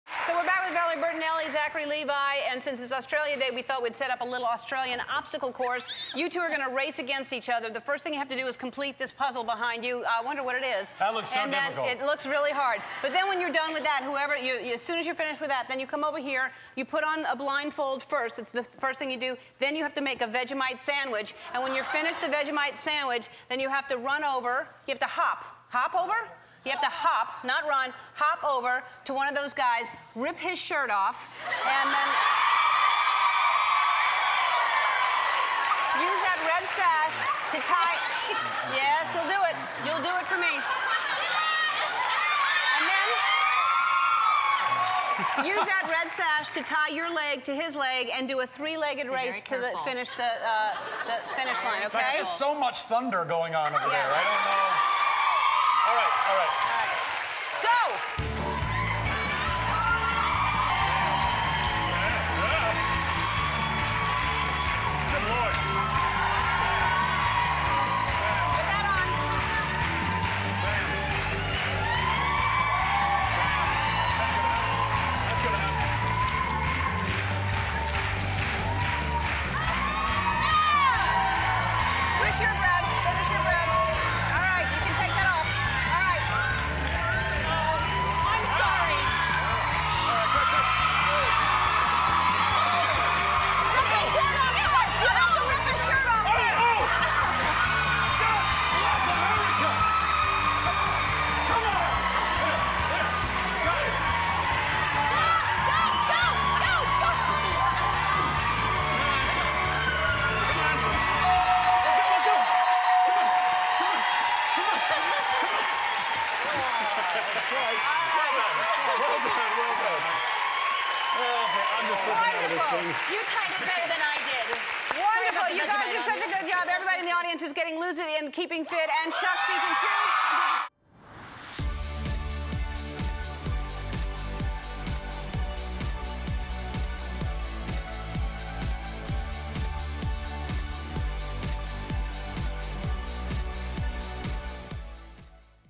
Australia Day Game with Zachary Levi and Valerie Bertinelli (Season 7)
[Original air date: January 23, 2010] Not to mention the Thunder from Down Under!